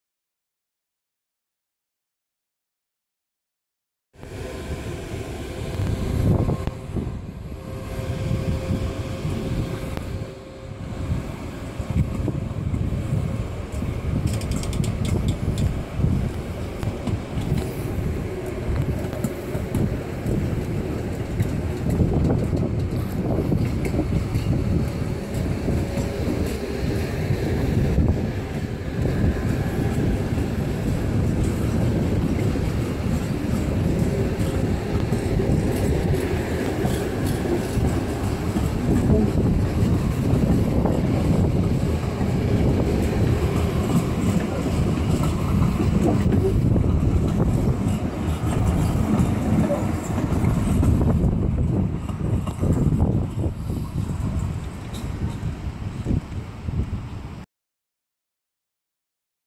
6 Car U2 Ctrain Shuttle To Haysbro Garage
A 6 car U2 consist on its way from Anderson garage to Haysbro storage.

Routinely everyday after the evening rush-hour trains get cleaned and serviced at Anderson garage.

A six-car mix contest (3 U2s+3 SD160) or a six-car U2 or 6 car SD160 consist makes its way from Anderson garage to Haysbro storage nice and clean ready to go back to work the next service day.

The service man simply uncouples the 6 car train when he arrives at Haysbro storage to make 2-3 car consists.

This saves time as the service man doesn't have to make 2 separate trips with two 3 car trains.